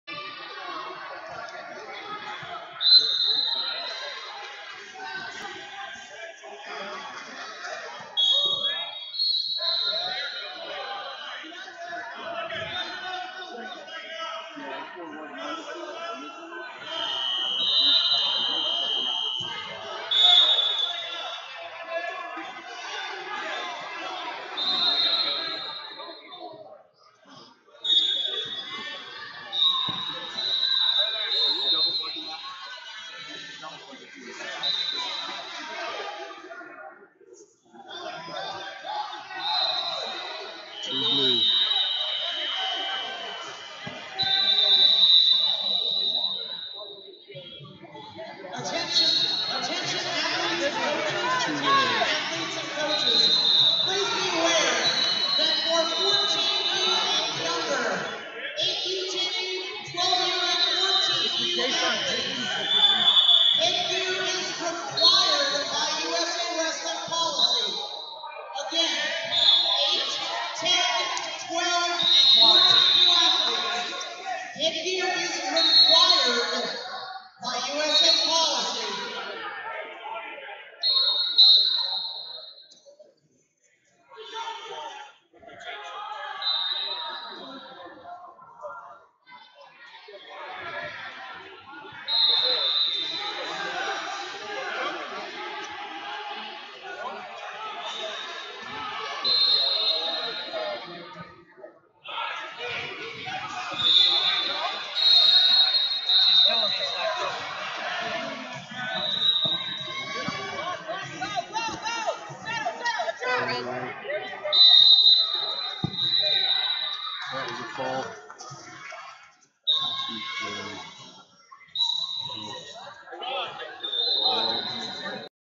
16U Boys 132 Chase Bell Black Fox Wrestling Academy Vs Brady Mason Mill Valley Kids Wrestling Club
2021 Southern Regionals GR - 16U Boys 132 Chase Bell Black Fox Wrestling Academy Vs Brady Mason Mill Valley Kids Wrestling Club